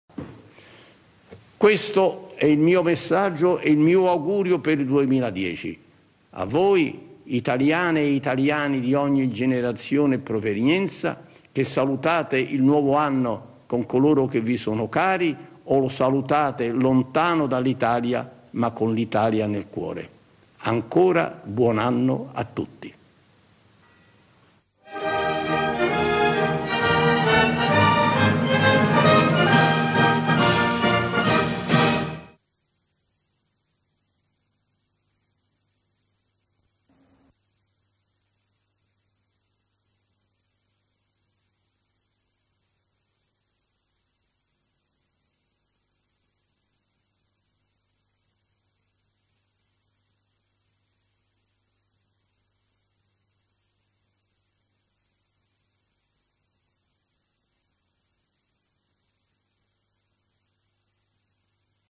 Fine discorso del presidente Napolitano e segnale orario - RaiUno ritrasmesso da TRS - 31-12-09
Ultima parte del discorso di fine anno del Presidente della Repubblica, trasmesso da RaiUno e grossolanamente ritrasmesso dall'emittente lombarda TRS, con seguente segnale orario dell'emittente. Registrato il 31/12/09.